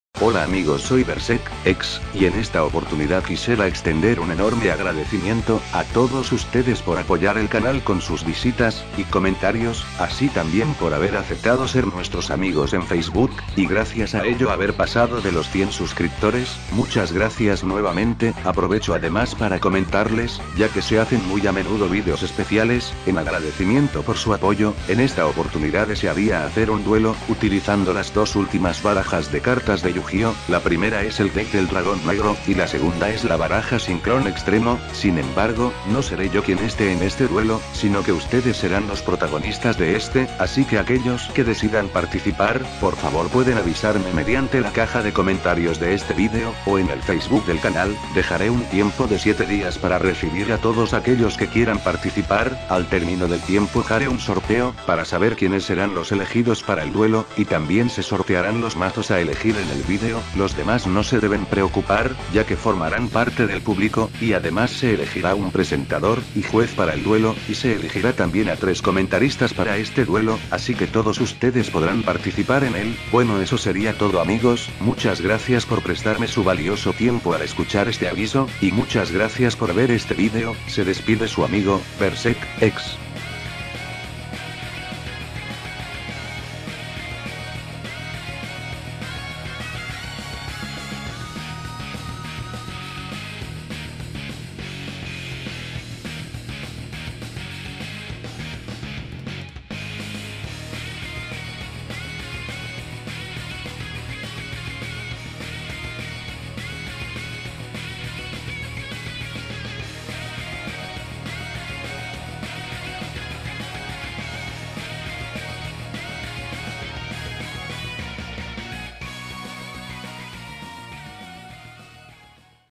AVISO: VIDEO ESPECIAL (TIEMPO TERMINADO HOY 12/10/2015)
SERA UN VIDEO DE UN DUELO ANIMADO ENTRE DOS DE USTEDES QUE SALGAN AL AZAR Y LOS DEMAS TAMBIEN PARTICIPARAN COMO COMENTARISTAS Y COMO PARTE DEL PUBLICO SERA UN VIDEO LOQUENDO DE DUELO YO LO ANIMARE EL DUELO PARA QUE SEA DIVERTIDO ANIMENSE AMIGOS